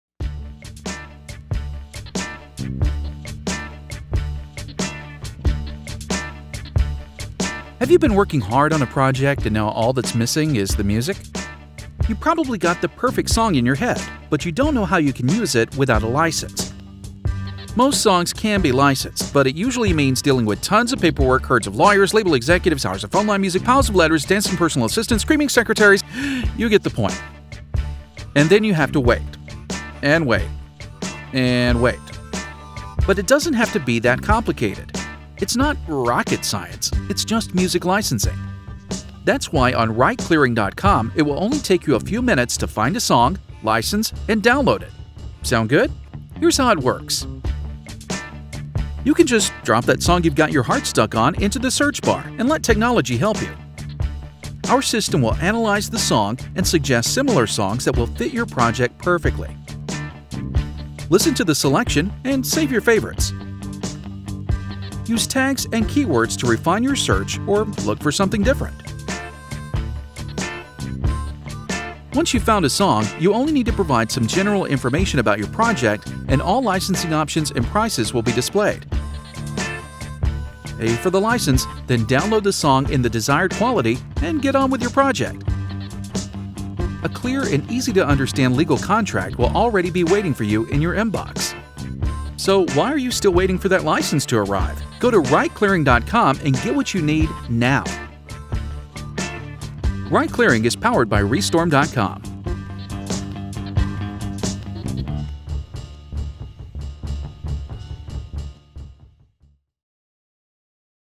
rightclearing.com - one-stop music licensing
rightclearing simplifies and democratises the market for music licensing. The whole process from finding the right songs to closing a legal contract and paying for the license has been completely automated.

Whether advertisers, filmmakers or producers of non-commercial content for YouTube - on rightclearing everyone can find the perfect songs for their project in no time.

Cutting-edge technology

Thanks to the innovative technology developed by the Fraunhofer Institute, all you need to do is upload a sound file and the system will quickly and automatically suggest matching songs.

Open & non-exclusive

Now also independent artists can license their music and thus get access to a new source of revenue. rightclearing is non-exclusive, so all rights to the songs remain with the rights holder.

Best conditions worldwide

Using rightclearing is free for artists and will remain so. Artists can define their own price plans and receive 100% of the price they set. rightclearing charges the licensee a low transaction fee of 10%.